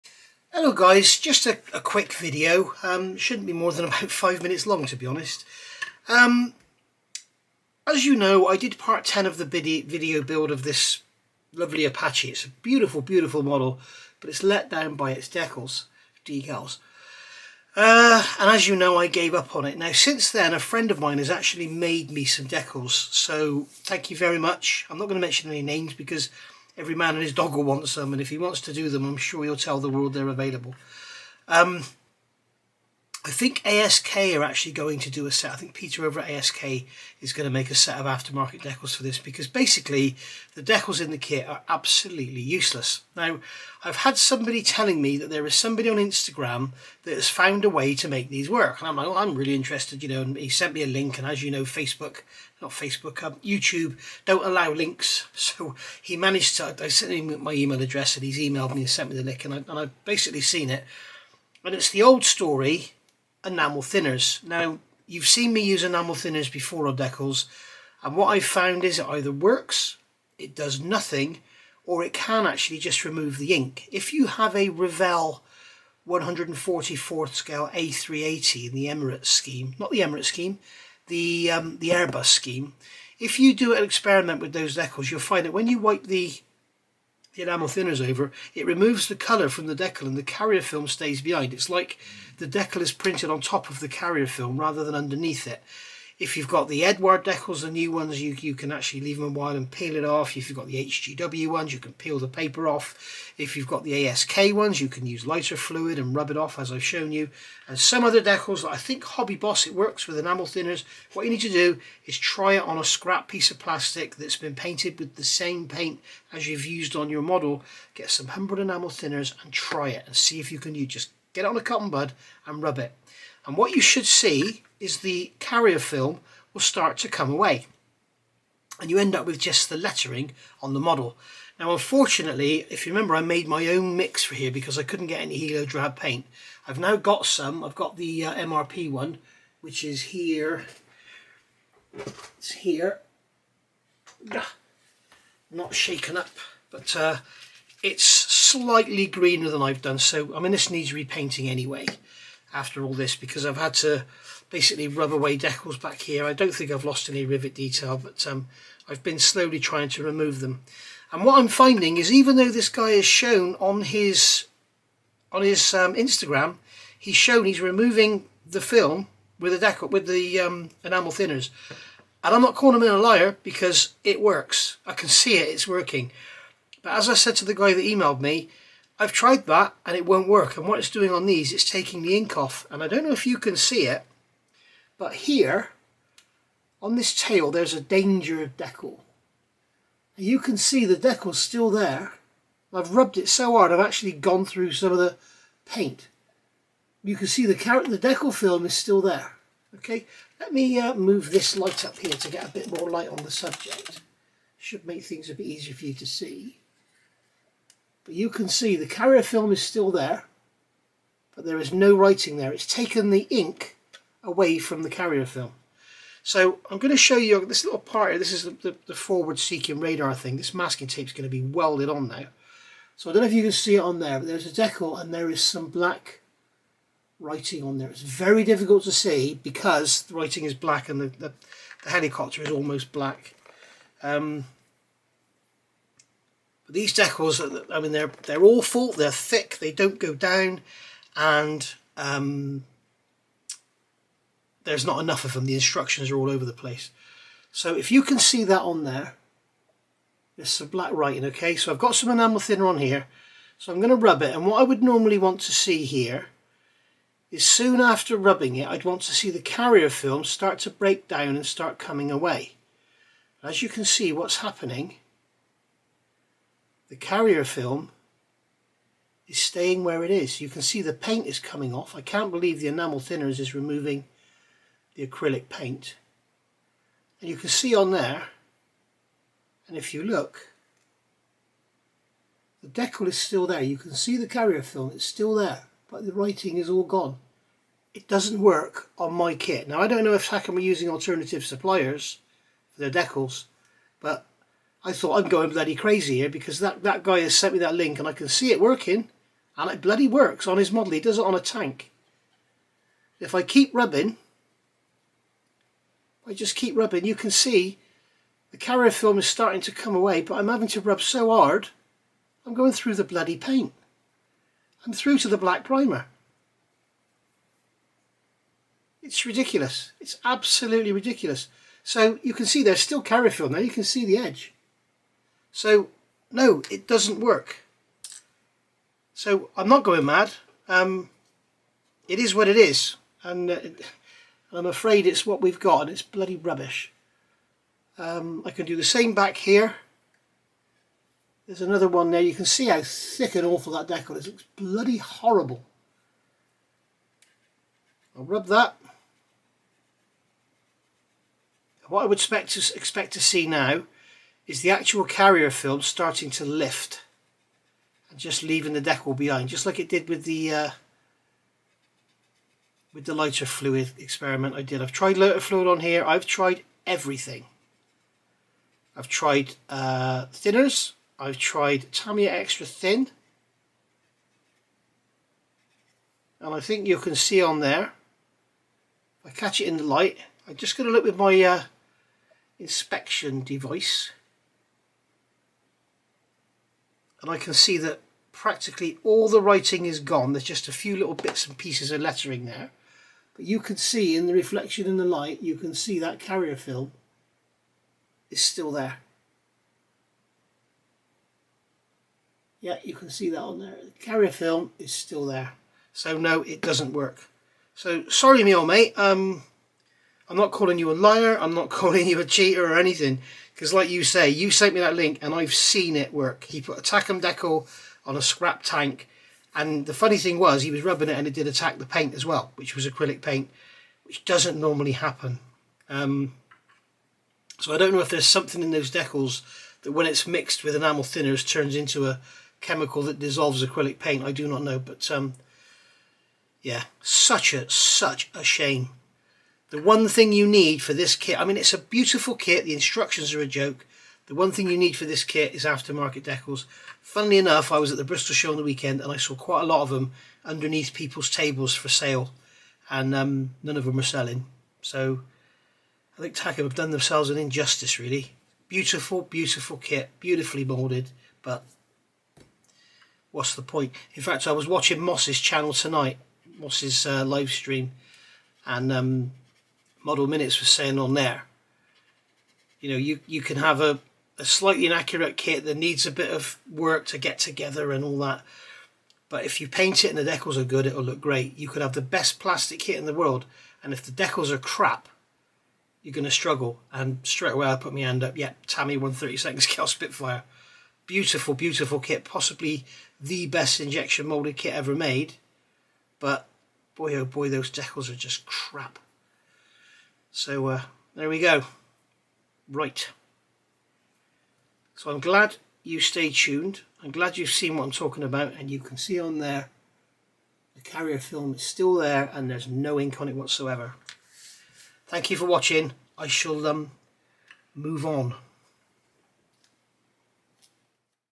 Takom 1/35 Apache decal update
I just want to show you all that the enamel thinner test does not work on my model, even though someone on Instagram has shown it working with their Decals? If you would like to contribute to the channel.. please look